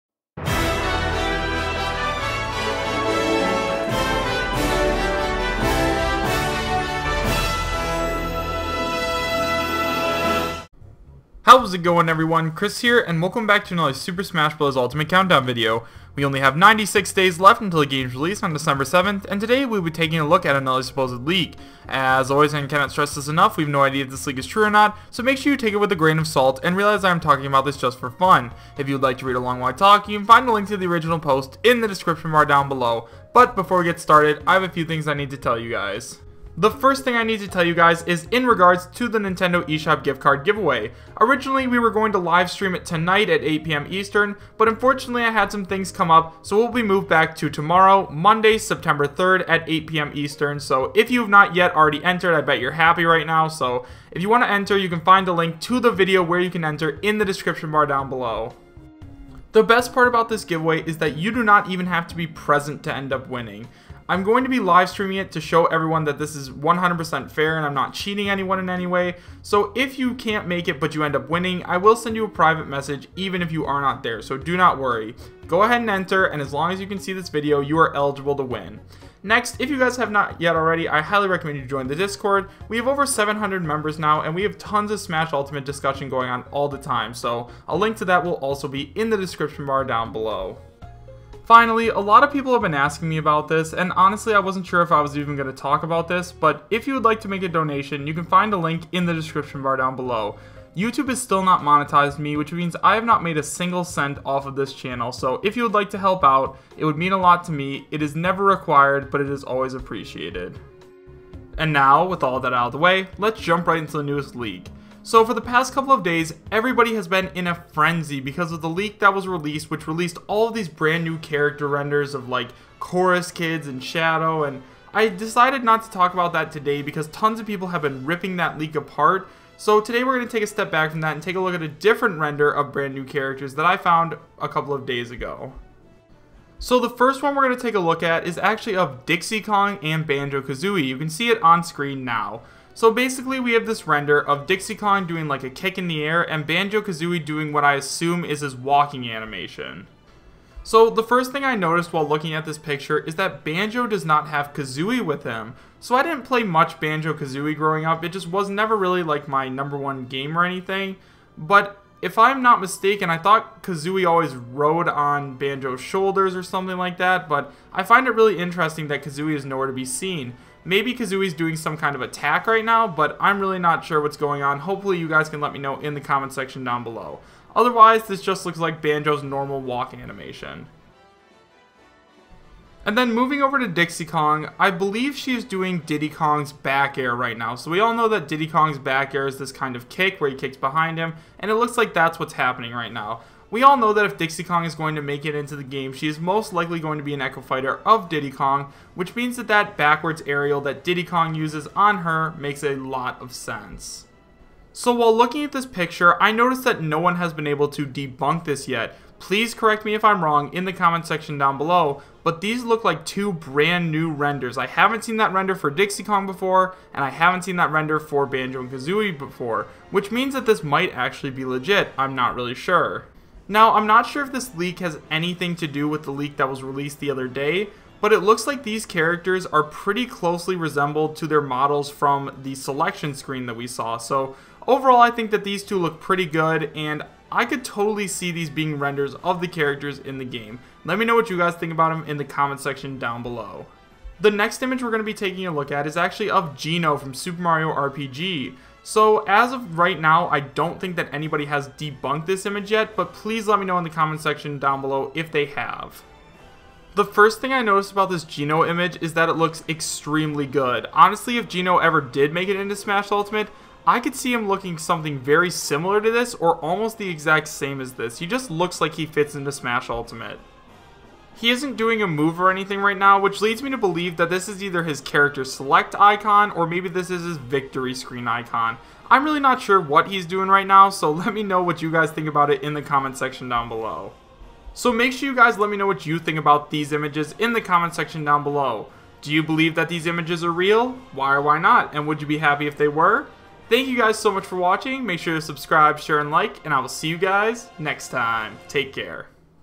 Smash Ultimate | AMAZING NEW CHARACTER RENDER LEAKS | THIS ONE LOOKS REAL!
Super Smash Bros Ultimate Countdown! 96 Days Remain!

Now that the Smash Ultimate Direct has come and gone, it's time to get back into the leak game!

I have been a fan of Super Smash Bros since the N64 days, and I have been playing ever since! I am a 21 year old Nintendo fan from Michigan, and I hope we can help each other pass the time until Super Smash Bros. Ultimate releases on December 7th!
-Chris